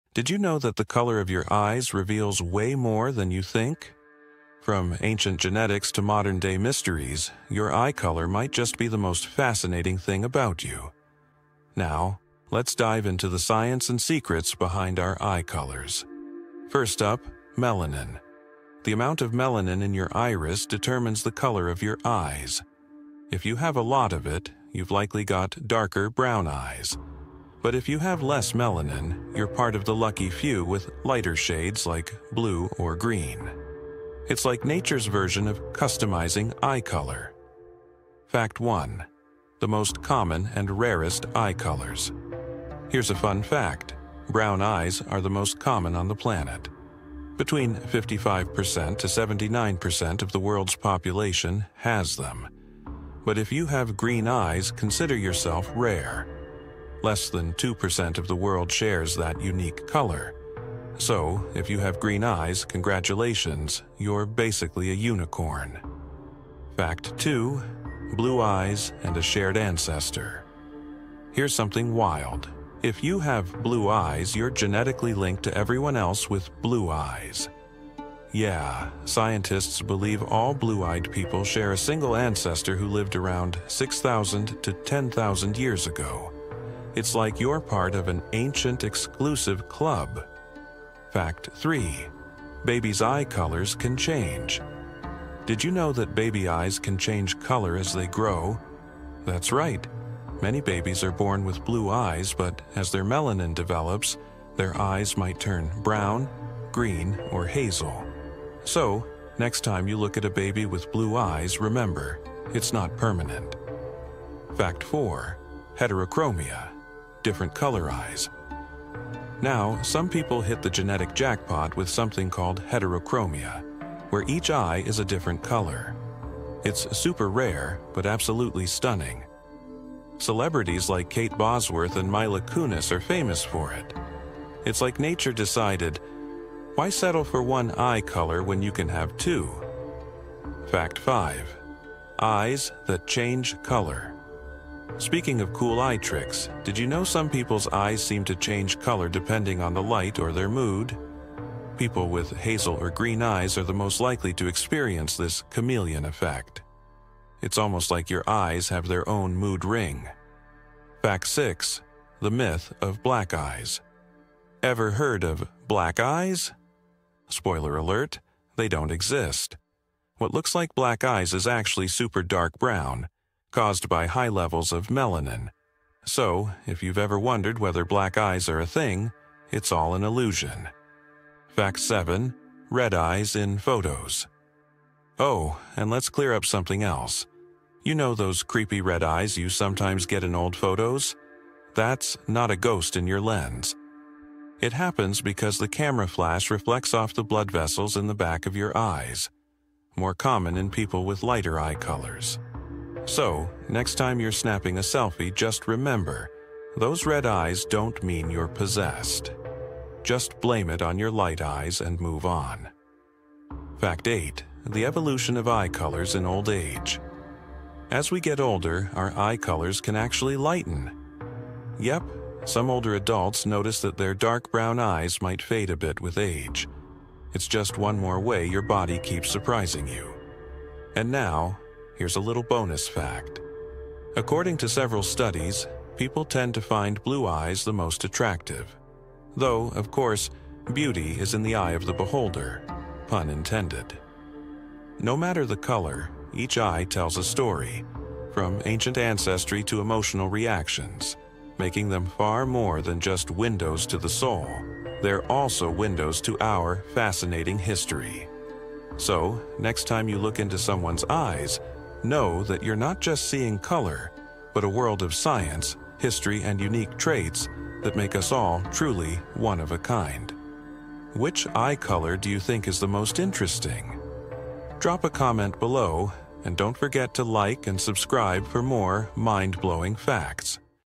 Eye Color Secrets That Will Shock You
Discover shocking eye color secrets! Learn what blue eyes, brown eyes, green eyes and rare eye colors reveal about genetics and melanin.

Did you know that your eye color is more than just a beautiful trait—it’s a window into your genetics, history, and even your personality? In this video, we dive deep into the science and secrets behind eye colors, from the role of melanin to the rarest shades like green and blue. Get ready to uncover some mind-blowing facts about your eyes! 👀✨

🔍 In this video, you’ll learn:

How melanin determines your eye color.

Why blue-eyed people share a common ancestor from 6,000 to 10,000 years ago.

The truth about green eyes—why they’re so rare!

Why babies’ eye colors can change over time.

The fascinating phenomenon of heterochromia (two different eye colors).

📌 Chapters:
0:00 - Introduction: The Fascinating World of Eye Colors
0:16 - How Melanin Determines Eye Color
0:42 - Fact 1: The Most Common & Rarest Eye Colors
1:10 - Fact 2: Blue Eyes and a Shared Ancestor
1:35 - Fact 3: Babies' Eye Colors Can Change
1:58 - Fact 4: Heterochromia – Different Color Eyes
2:20 - Fact 5: Eyes That ‘Change’ Color
2:44 - Fact 6: The Myth of Black Eyes
3:04 - Fact 7: Red Eyes in Photos
3:37 - Fact 8: The Evolution of Eye Colors in Old Age
3:59 - Bonus Fact: Are Blue Eyes the Most Attractive?